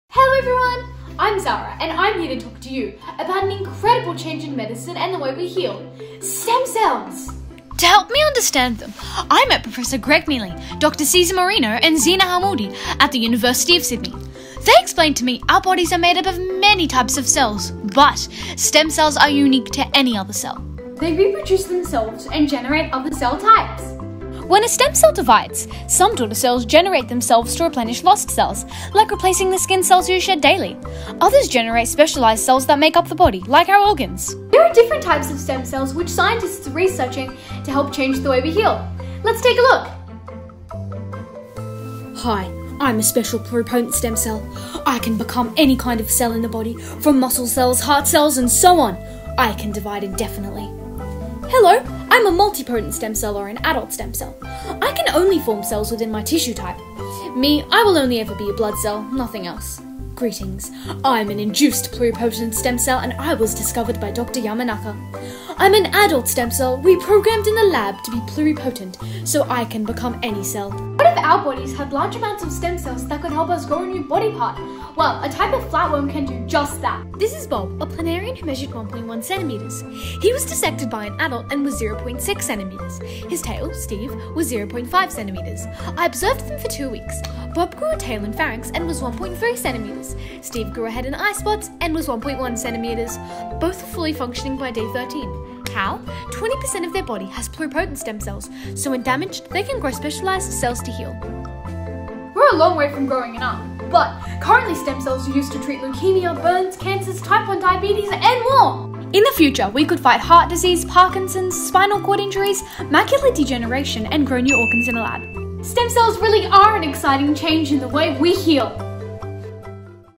Stem Cells: Changing the Way We Heal - Third Place, 2022 Sleek Geeks Science Eureka Prize – Primary
Zara M., NSW

Third Place, 2022 University of Sydney Sleek Geeks Science Eureka Prize - Primary

The information contained in this video represents the views and opinions of the original creators and does not necessarily represent the views or opinions of the Australian Museum.